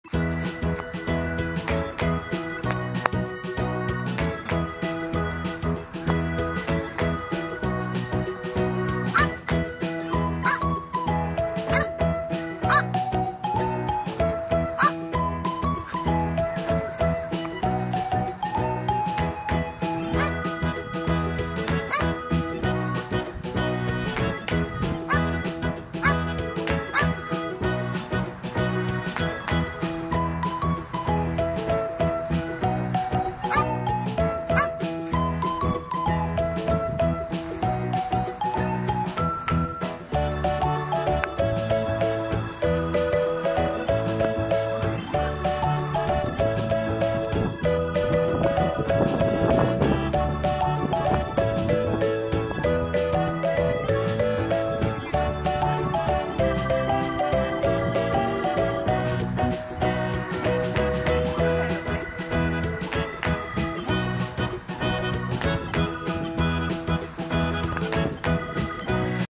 Cardi Corgi wants to play with rat pals!
Video taken 29 September 2017! Cardi Corgi is back!